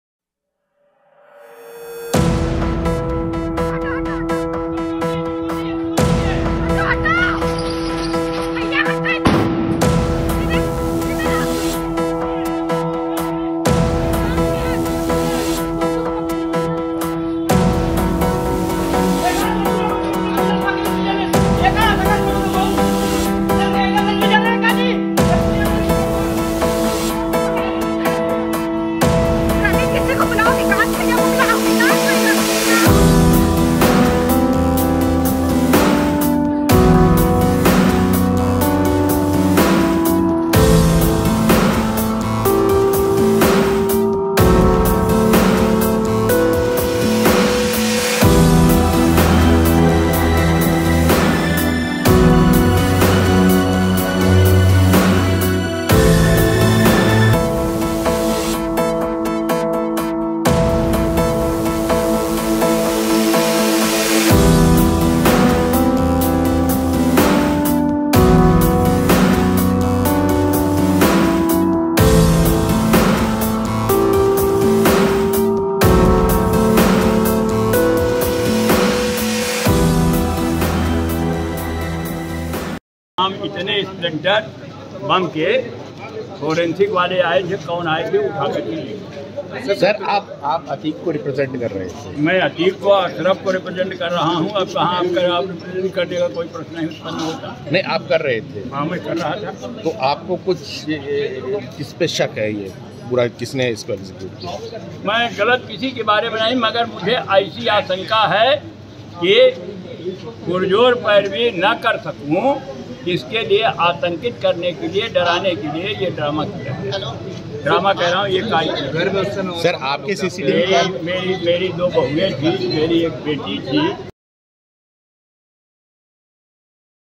Atique Ahmed के वकील की गली में फटा देसी बम, Blast पर क्या बोली Prayagraj Police ? । UP Crime News
Atique Ahmed News: प्रयागराज में गैंगस्टर अतीक अहमद के वकील के घर के पास देसी बम फेंका गया. इस घटना से लोगों में दहशत का माहौल है. हालांकि इस घटना में किसी की हताहत होने की खबर नहीं है. अतीक अहमद के वकील दयाशंकर मिश्र का आरोप है कि यह घटना आतंकित करने के लिए की गई है. वहीं पुलिस कमिश्नर ने बयान जारी कर कहा कि दूसरी मामले को अतीक के वकील के घर पर बम फेंकने की घटना से जोड़ा जा रहा है. जांच में आया कि दो पक्षों के आपसी विवाद में बम फेंका गया है. मौके पर कोई हताहत नहीं है. पूरे मामले को लेकर कार्रवाई की जा रही है. अतीक अहमद के वकील के घर पर हमले की बात पूरी तरह असत्य है.

Bomb Attack in Prayagraj: A crude bomb was hurled near the house of the lawyer of gangster Atiq Ahmed in Prayagraj. Due to this incident, there is an atmosphere of panic among the people. However, there is no news of any casualty in this incident. Atiq Ahmed's lawyer Dayashankar Mishra alleges that this incident has been done to terrorize. At the same time, the police commissioner issued a statement saying that the second case is being linked to the incident of throwing a bomb at Atiq's lawyer's house. It came to the investigation that the bomb was thrown in a mutual dispute between the two parties. There is no casualty on the spot. Action is being taken regarding the whole matter. The talk of attack on Atiq Ahmed's lawyer's house is completely untrue.